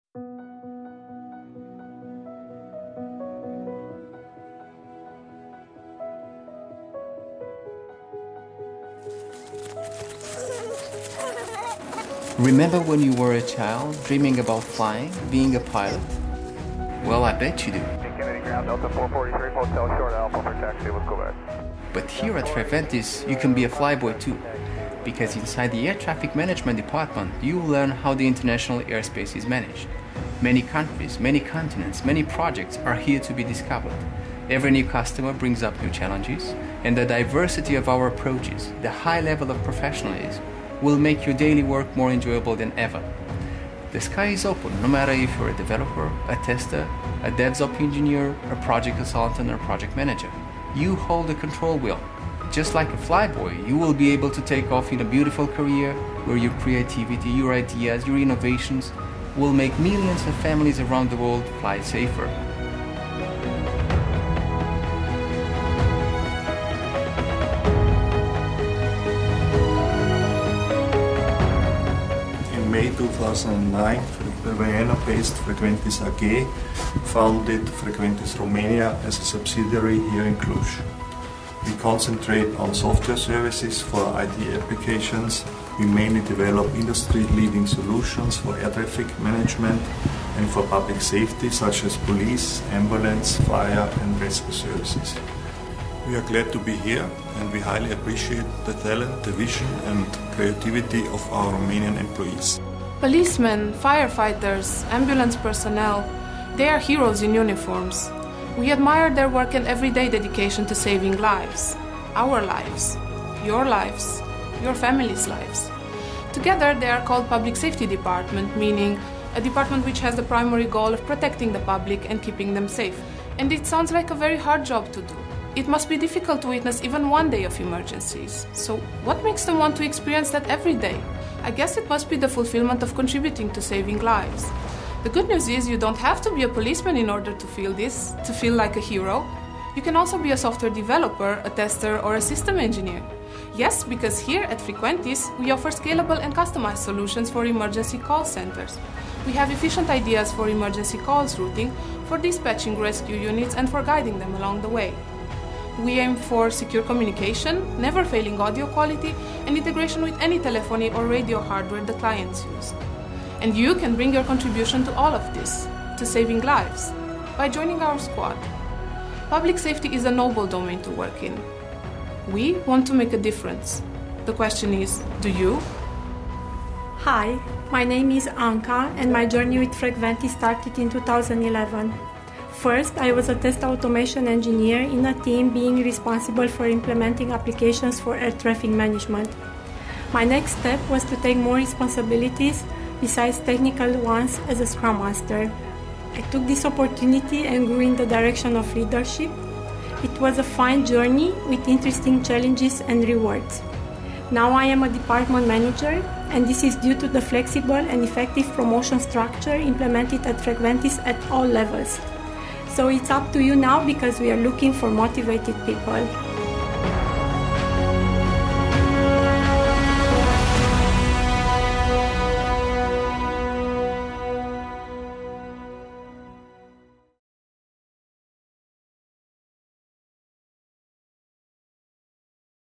Follow your talent in Air Traffic Management and Public Safety. (Frequentis Romania)
Presenting the activities and the team from the Cluj – Napoca subsidiary: " Here, in Cluj, we are concentrating on software services in the field of IT applications, developing industry-leading solutions mainly for Air Traffic Management (ATM) and for Public Safety (police, ambulance and fire & rescue services)"